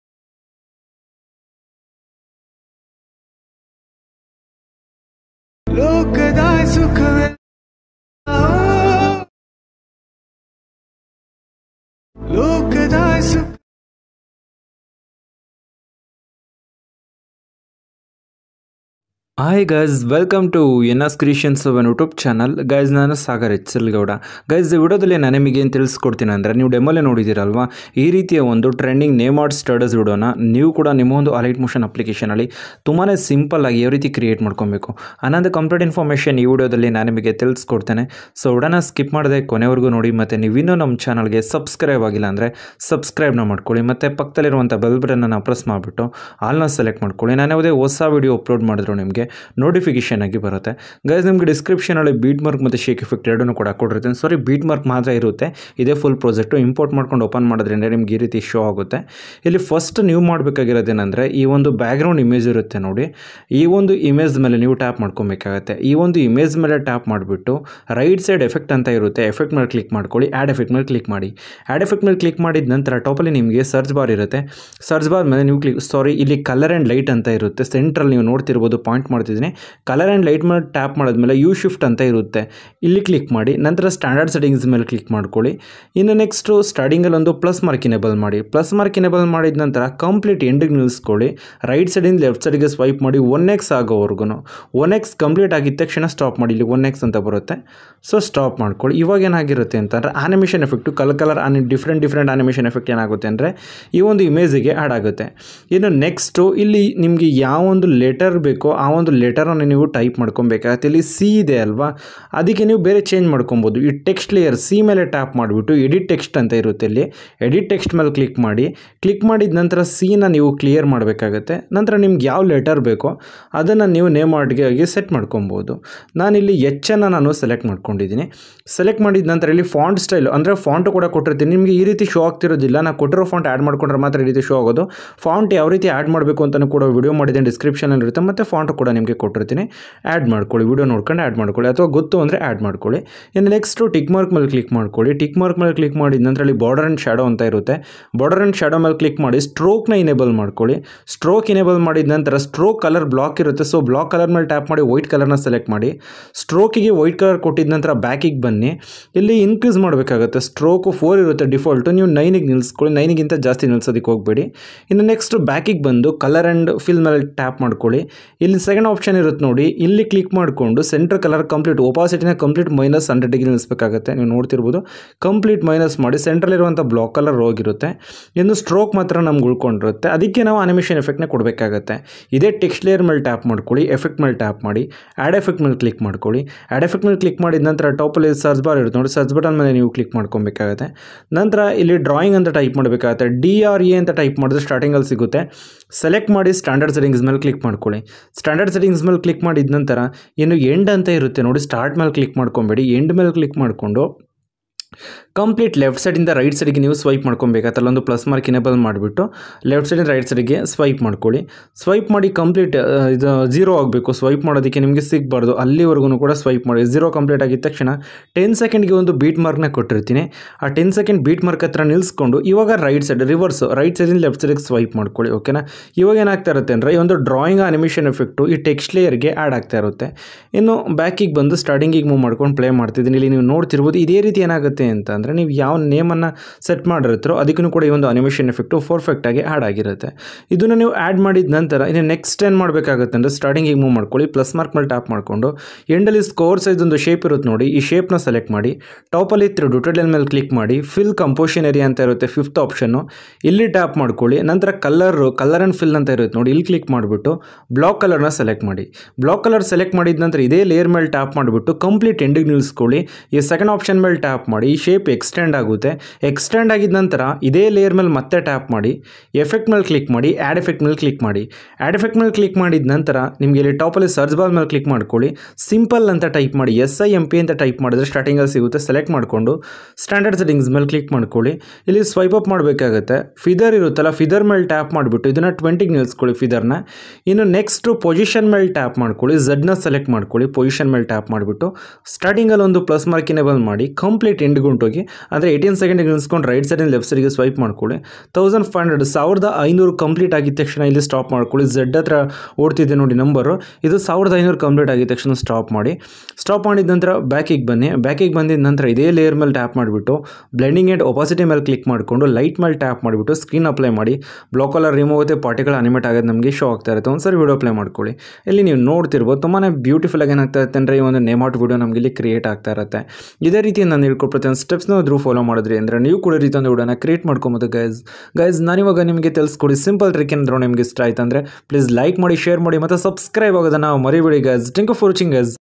Instagram new trending name art reels video editing tutorial | kannada | new style name video edit
⚡BEAT MARK ⚡(FULL PROJECT)